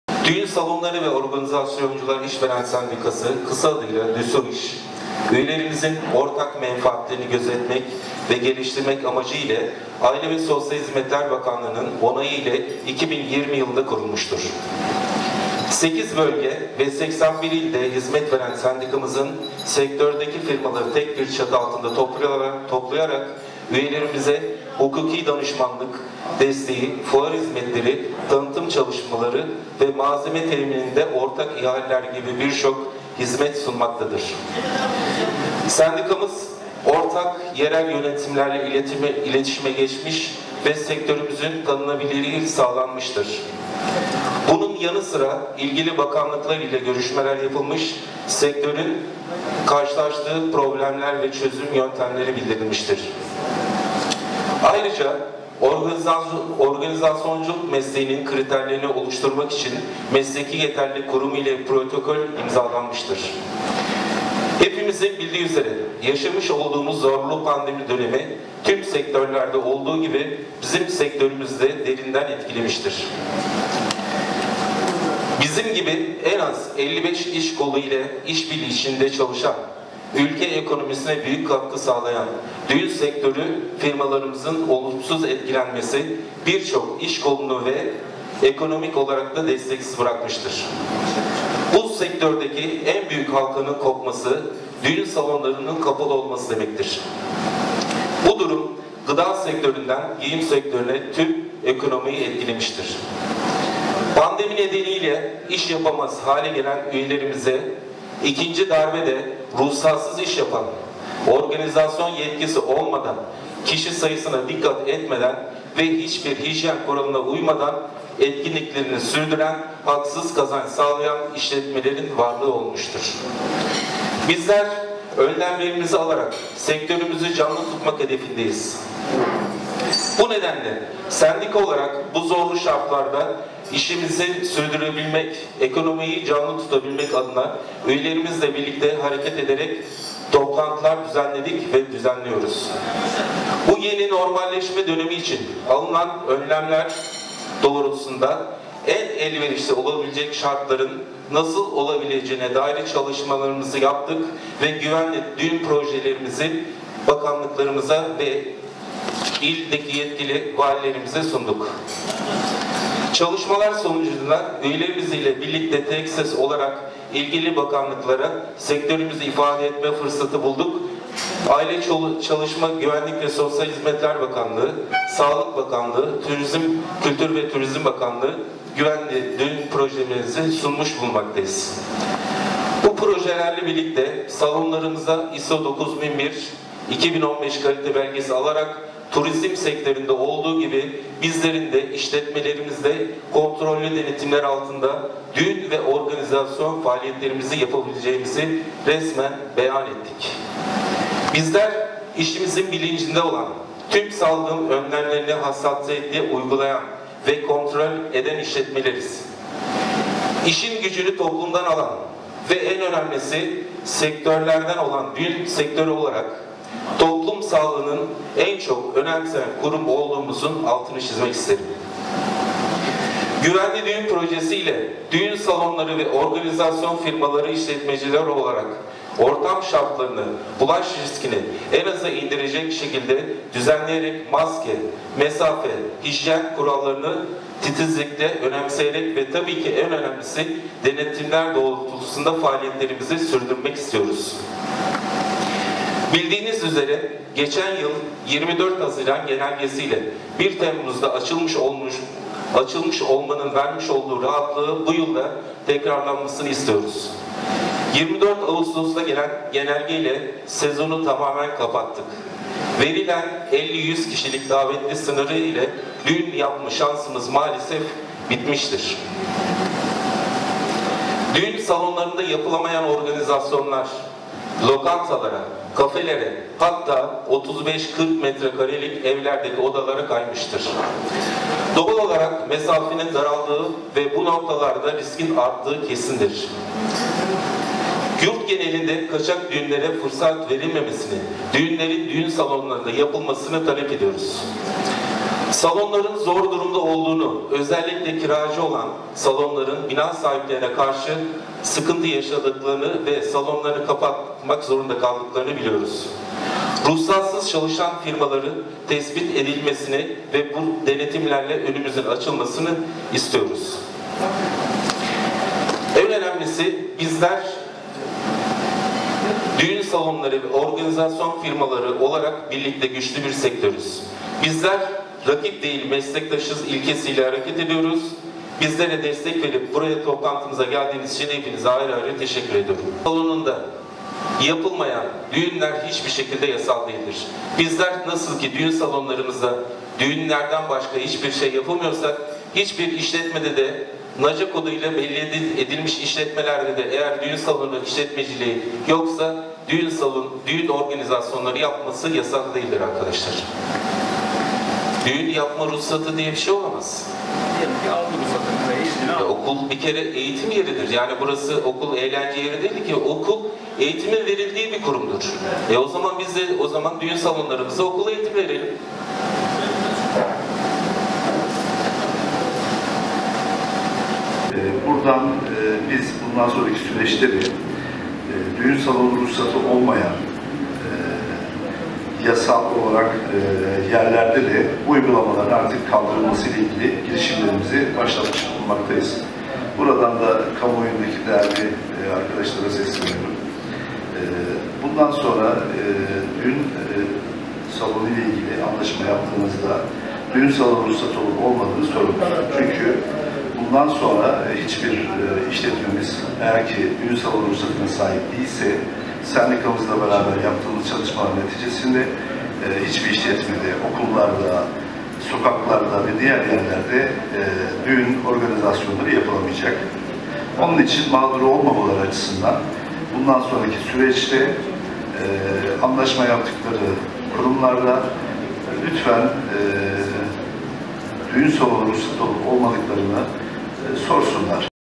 Ruhsatlı Olmayan Okul Salonları Düğün Yapamayacak
2020 yılında kurulan Düğün Saloncuları ve Organizasyoncular İşveren Sendikası (DÜSOİŞ)’na Ünye’de Düğün Salonu ruhsatı bulunan 6 işletme katıldı. Ordu’da ilk olarak Ünye’de sendikal faaliyetlere başlanırken Sendika Temsilciliğine İş İnsanı Mustafa Darıoğlu getirildi.
Darıoğlu “Ünye’de Güvenli Düğün Projesini Hayata Geçireceğiz”
DÜSOİŞ’in Ünye’de yapılan ilk toplantısında 2021 Düğün sezonu değerlendirildi. Pandemi döneminde yaşanan sıkıntıların ve çözüm önerilerinin masaya yatırıldığı toplantıda bir açıklama yapan DÜSOİŞ Ünye Temsilcisi Mustafa Darıoğlu “DÜSOİŞ sendikası bugün sorunlarımızı, pandemi döneminde yapacağımız çalışmaları değerlendirmek adına toplantı gerçekleştirdik. Biz kendi sektörümüz olarak pandemi döneminde kurban seçilen sektörlerden bir tanesiyiz. Tabi hepimiz bireysel olarak sorunları problemleri anlatıyoruz ancak sesimiz çevremizden dışarı çıkamıyor. Artık sendikamız, Ankara’da sesimiz olacak. Biz ruhsatlı düğün salonları olarak pandemi döneminde tüm hijyen kuralları, maske ve mesafelere dikkat ederek düğünler gerçekleştirdik. Ancak herkes düğünlerde düğün salonlarında koronavirüs bulaştı şeklindeki haberleri sıkça gördü. Aslında o haberler bizim gibi ruhsatlı düğün salonlarında tüm tedbileri alan işletmelerde değil, sokakta ve ruhsatsız yerlerde pandemi kuralları uygulanmayan düğünlerde vakalar görüldü. 2020 yılında Ünye’de gerçekleştirilen düğünlerde biz salon girişlerinde liste tuttuk ve ruhsatlı salonlarda bir tane vaka görülmedi. Bu nedenle 2021 yılı sezonu için Güvenli Düğün projesini önemsiyoruz ve hayata geçireceğiz. Pandemi sürecinde güvenli bir şekilde düğünlerimizi yapmaya devam edeceğiz” diye konuştu.